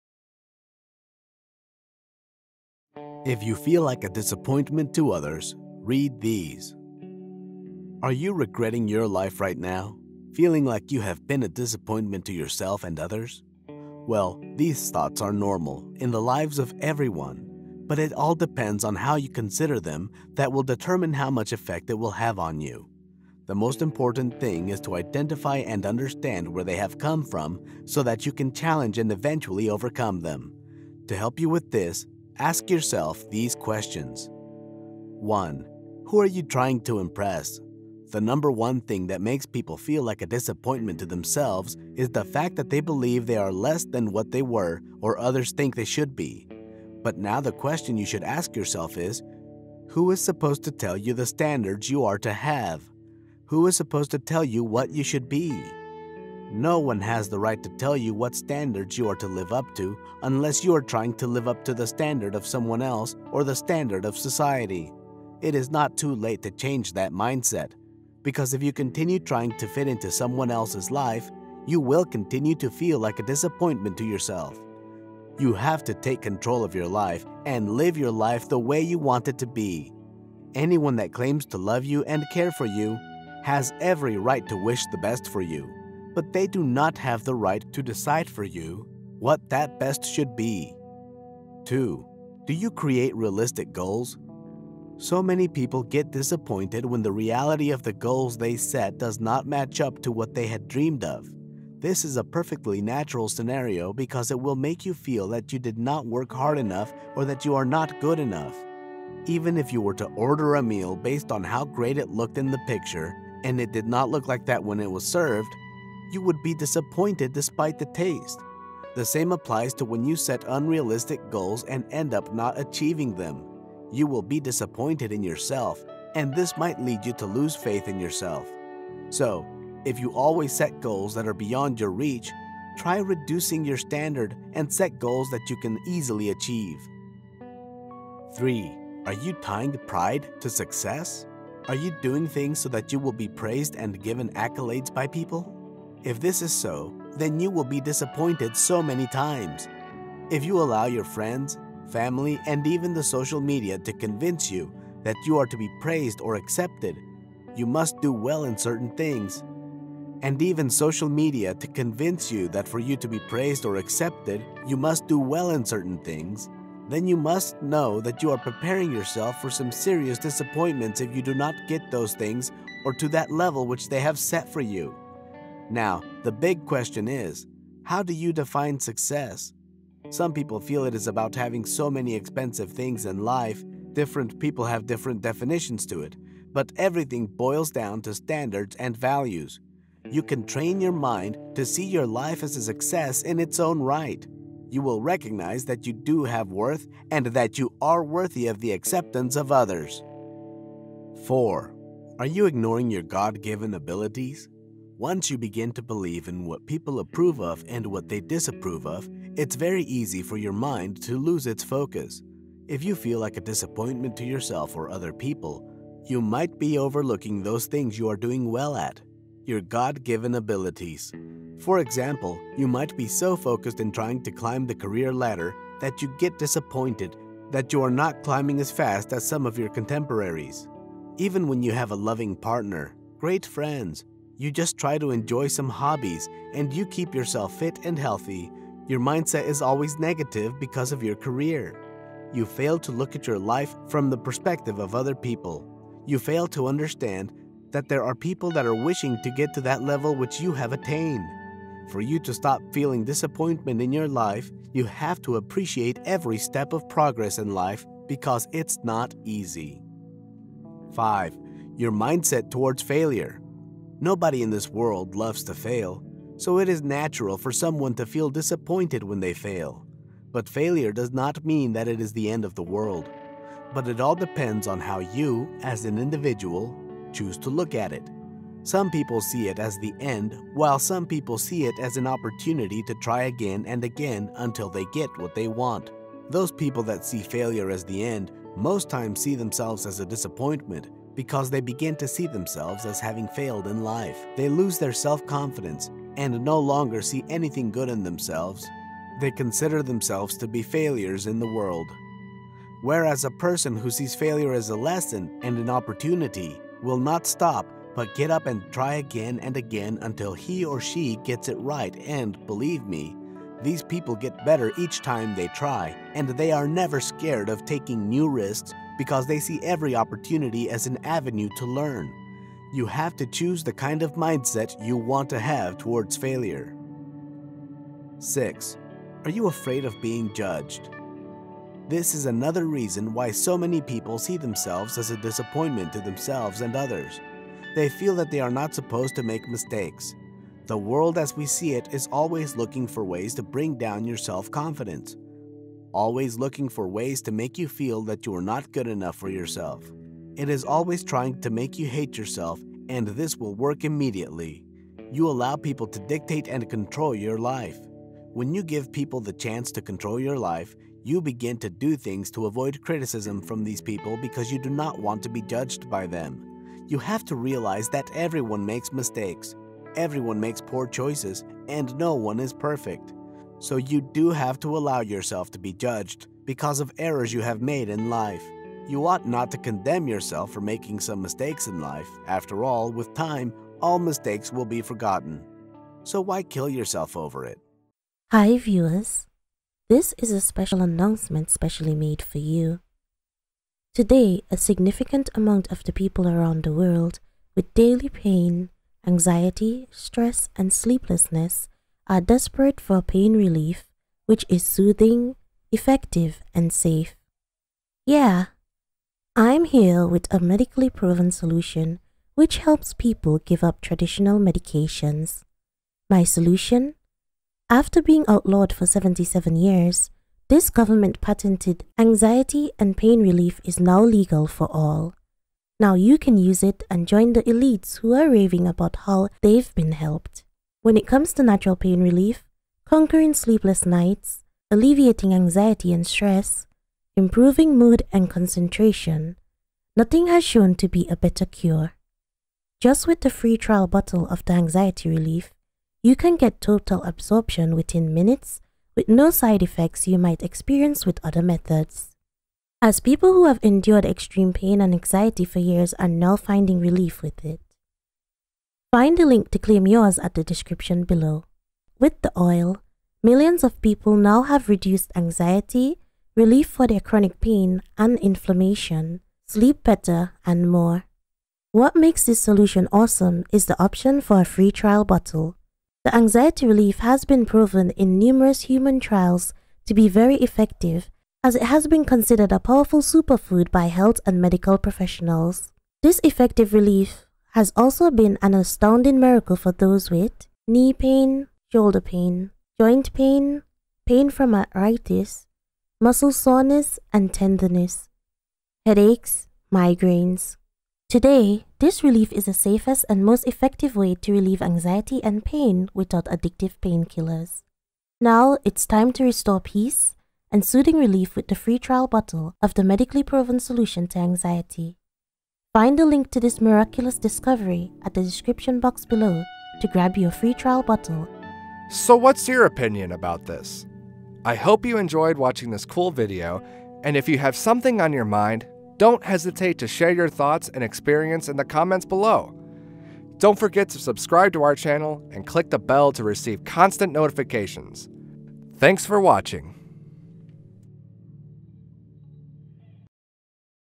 If You Feel Like a Disappointment to Yourself or Others, Watch These
Are you regretting your life right now, feeling like you have been a disappointment to yourself and others?
Well, these thoughts are normal in the lives of everyone, but it all depends on how you consider them
that will determine how much effect it will have on you. The most important thing is to identify and understand where they have come from
so that you can challenge and eventually overcome them. To help you with this, ask yourself these questions.